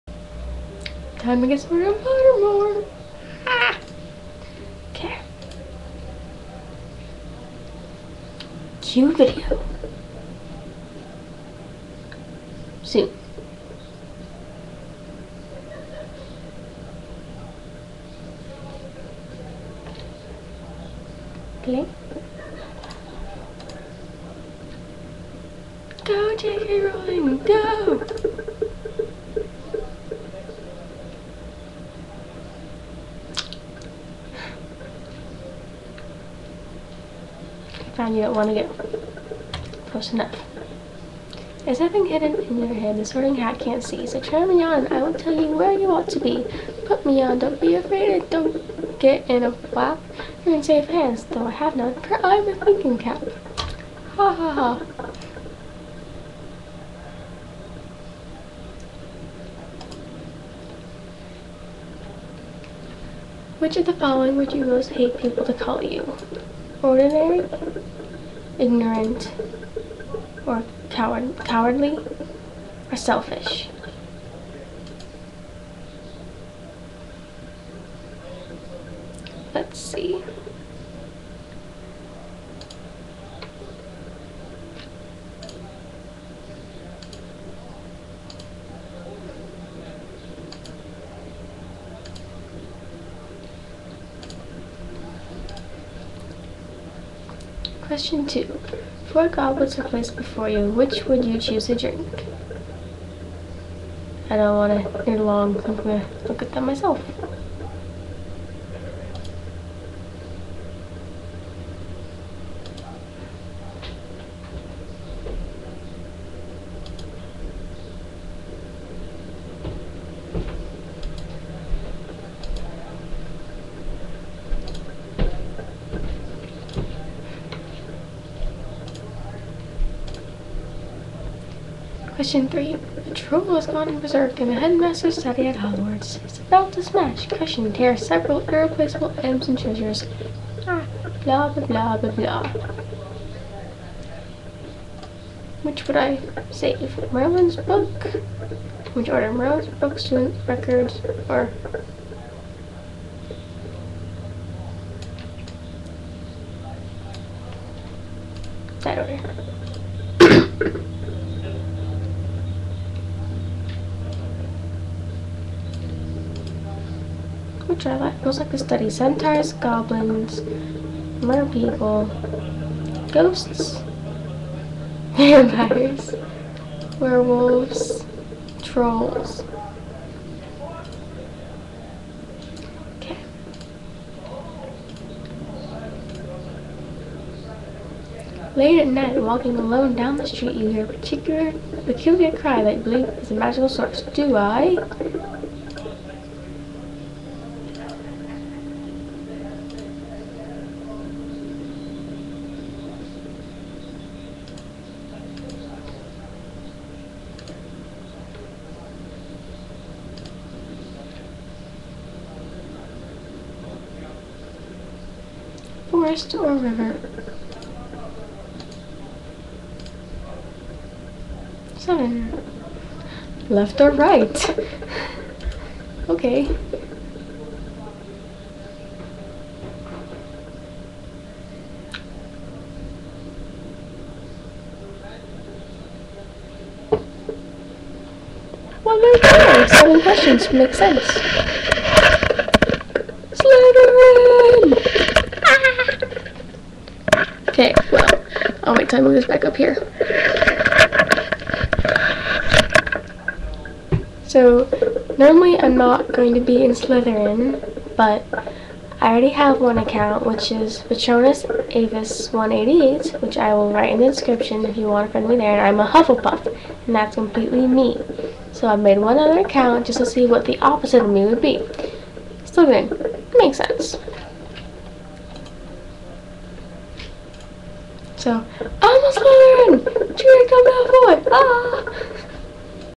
Pottermore Sorting Hat!
So as i explain at the end, this is my 2nd account, the first is the true me, and i'm a hufflepuff at PatronusAvis188.

With this account, i'm a ___..you'll just have to watch to see :P
and my username is RiverSky14603